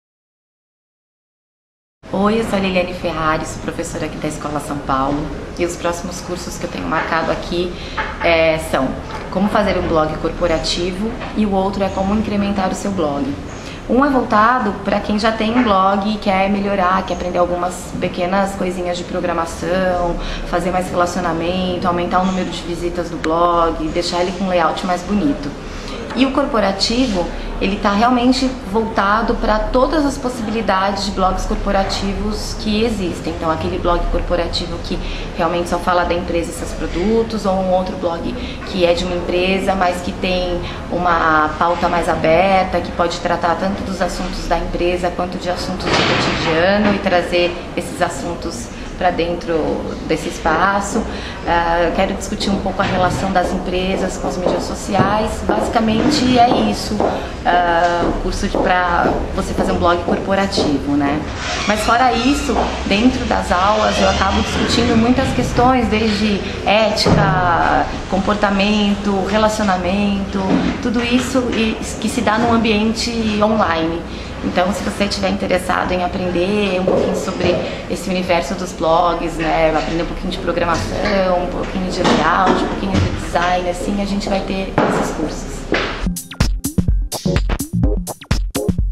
Curso: Incremente seu Blog, com Lili Ferrari
Conheça os cursos Incremente seu Blog e Como Fazer um Blog Corporativo ministrados pela blogueria e social media Lili Ferrari, na Escola São Paulo.